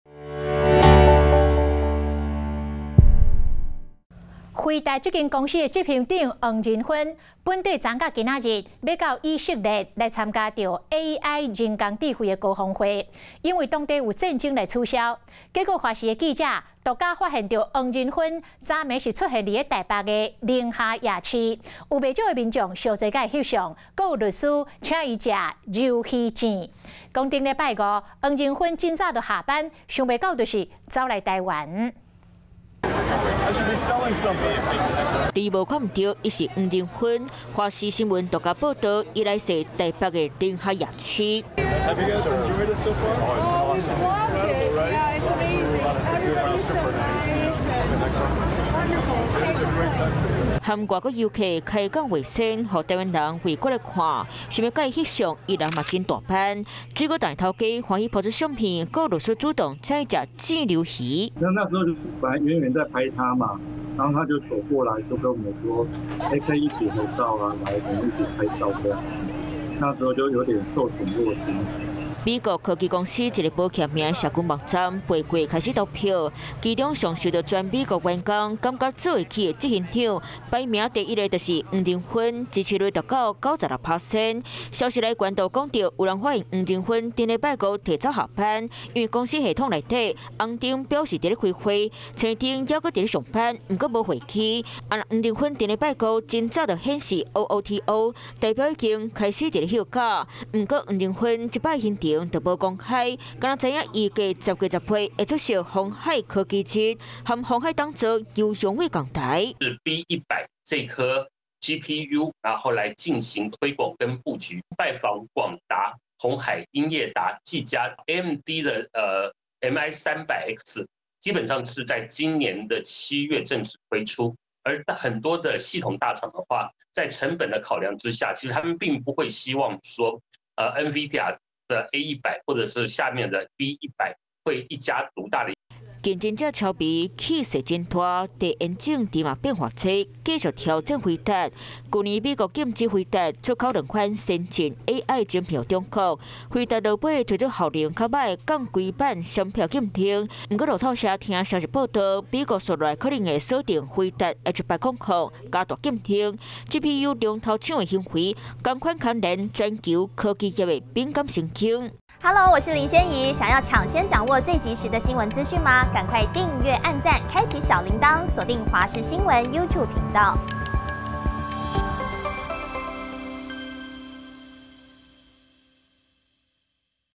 獨！黃仁勳現身寧夏夜市 民眾搶合照.請吃炸魷魚｜華視台語新聞 2023.10.16
輝達執行長黃仁勳，受以巴衝突影響，取消原本昨(15)日和今(16)日要在以色列舉辦的輝達AI高峰會，但華視新聞獨家捕捉，他昨日晚間現身台北寧夏夜市，親民地和民眾合照互動，還吃了豆花、炸魷魚等小吃。有消息來源也透露，上周五黃仁勳很早就下班，沒想到就是跑來台灣，黃仁勳後天也將出席鴻海科技日，雙方是否會有進一步合作，備受關注。